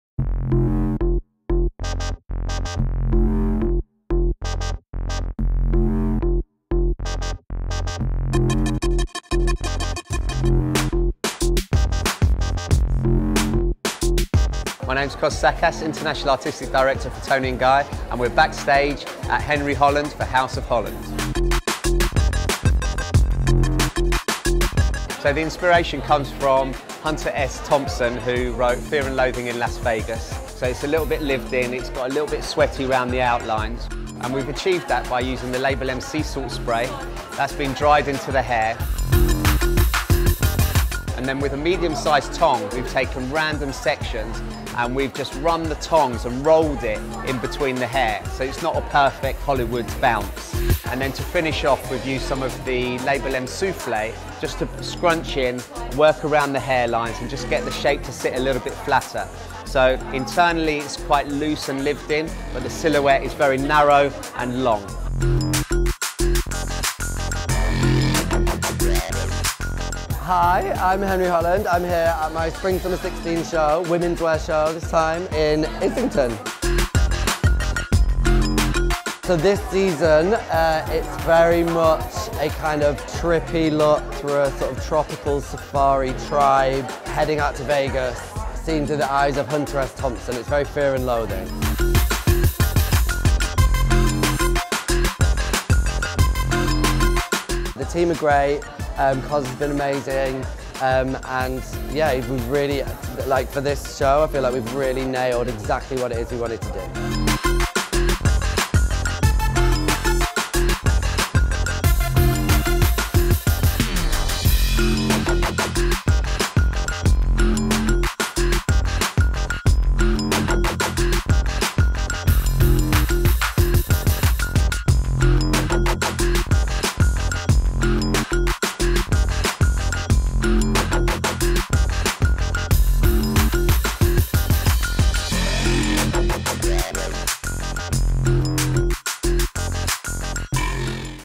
label.m Backstage at House of Holland LFW SS16
Backstage at House of Holland with International Artistic Director Cos Sakkas for TONI&GUY at London Fashion Week spring summer 2016